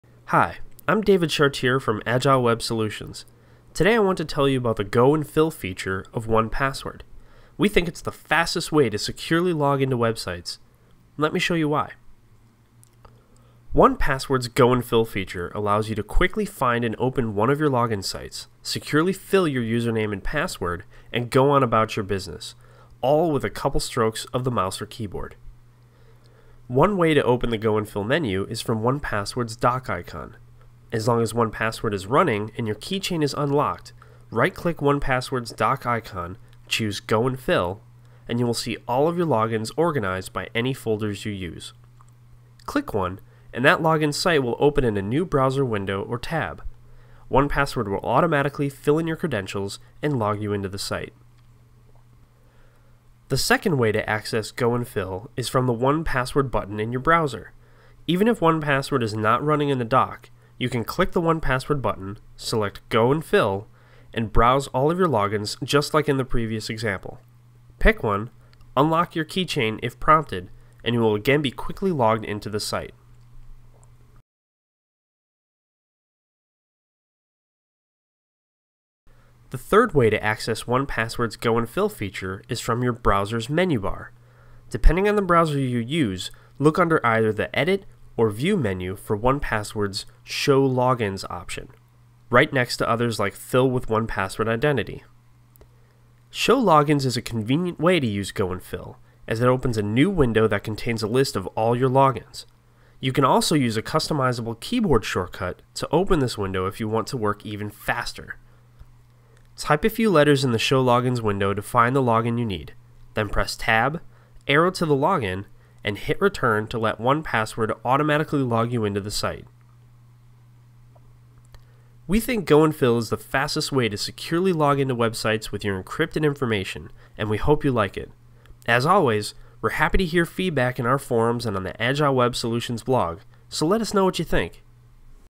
1Password Screencast: Go & Fill
1Password's Go & Fill feature allows you to quickly find and open one of your Login sites, securely fill your username and password, and go on about your business—all with a couple strokes of your mouse or keyboard. Here's how to use it.